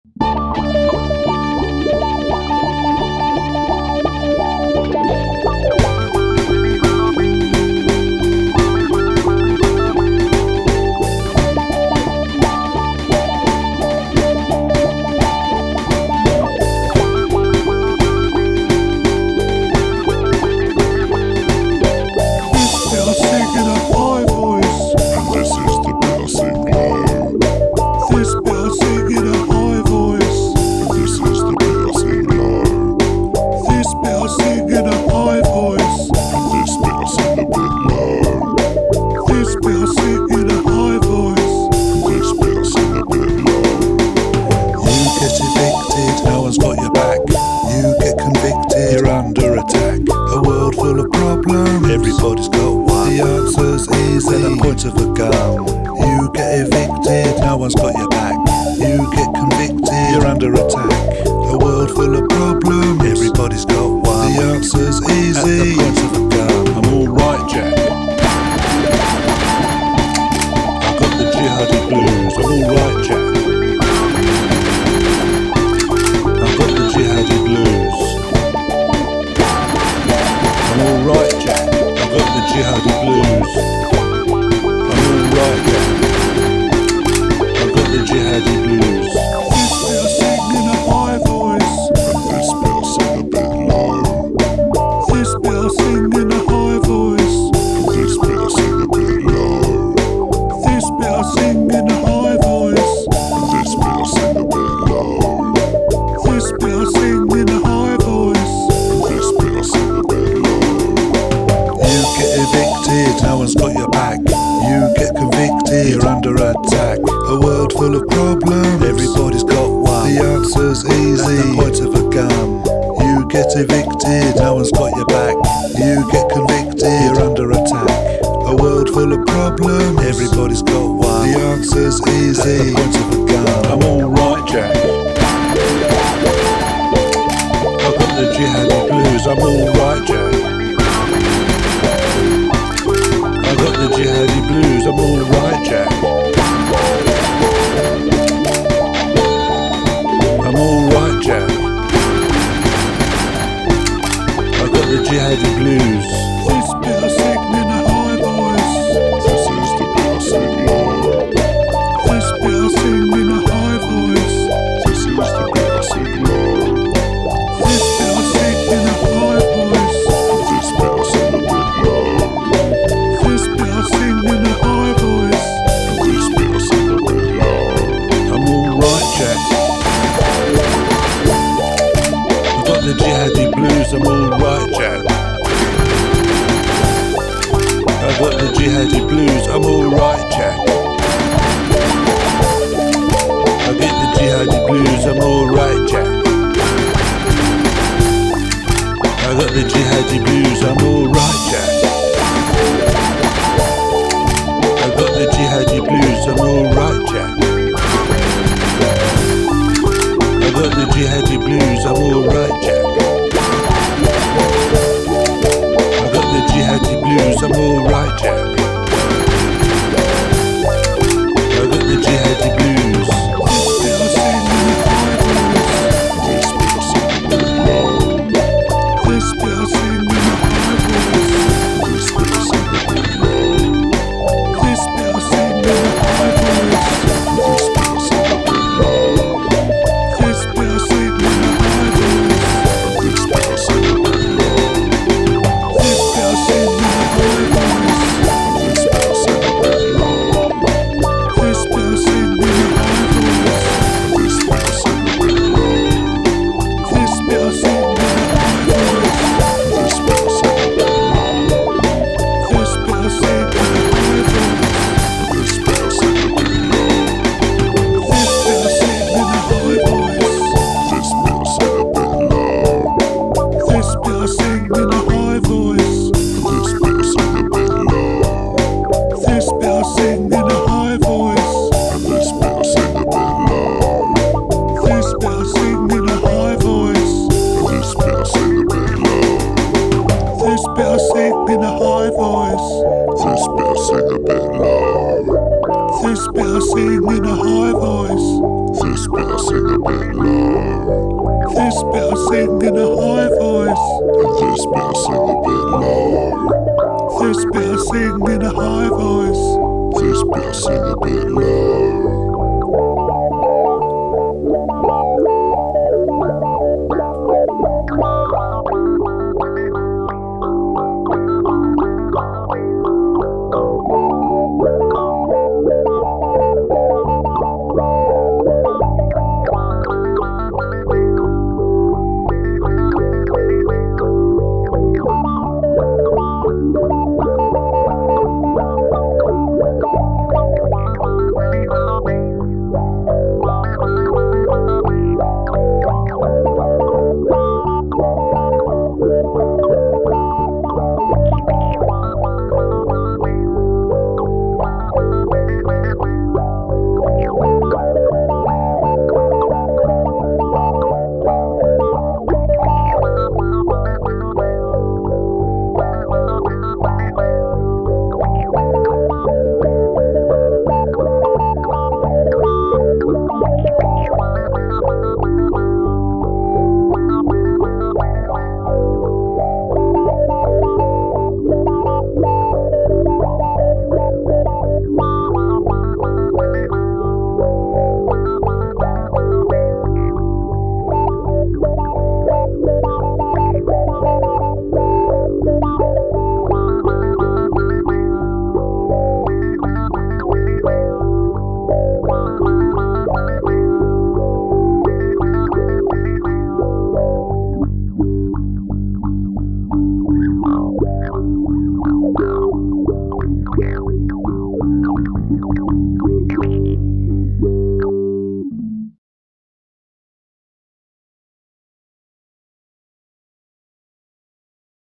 Jihadi Bloos
I wondered what it would be like if you lost everything. Then with nothing more to lose, you find a gun.